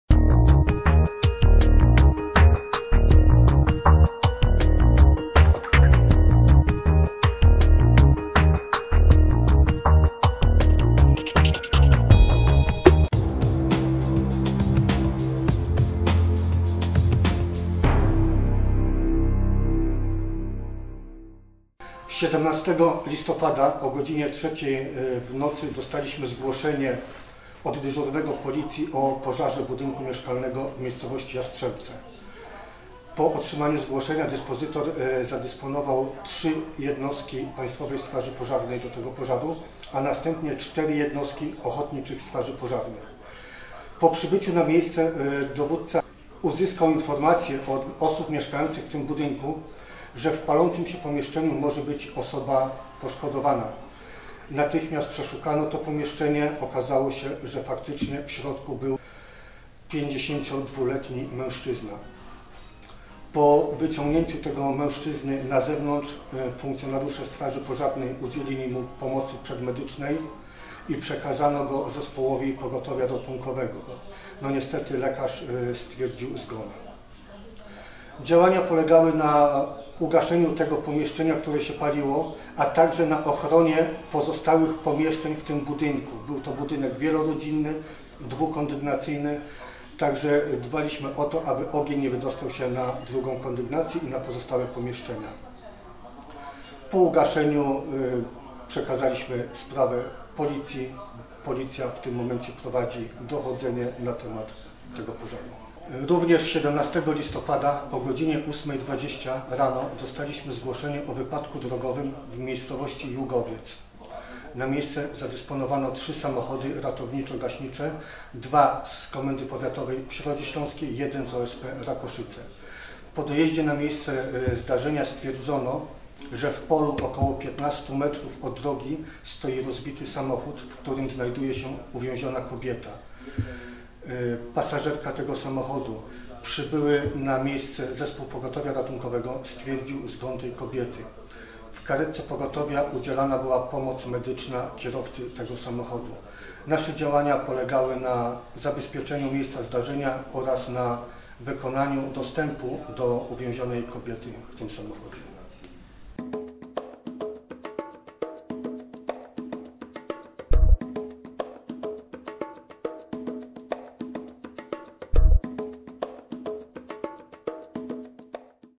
TRAGICZNA SOBOTA
W poprzednią sobotę na terenie gminy Środa Śląska wydarzyły się aż dwa tragiczne wypadki, w których śmierć poniosły dwie osoby.